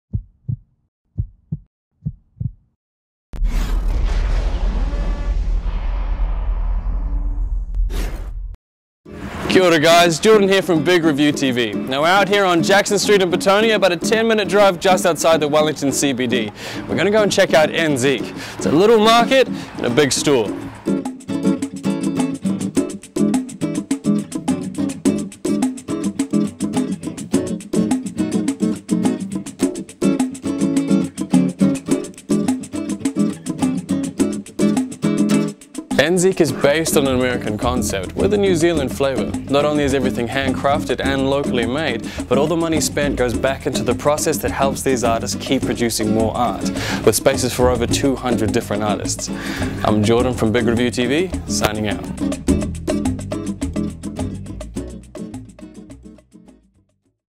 NZique, a Gift Shop in Wellington selling Gift Items, Souvenirs, Art and Craft
NZique is the home of thousands of unique, New Zealand-made gifts, souvenirs and works of art. Everything they sell has been made in NZ, by one of the many talented craftspeople and artists they represent.

N Zique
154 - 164 Jackson St Wellington New Zealand 5012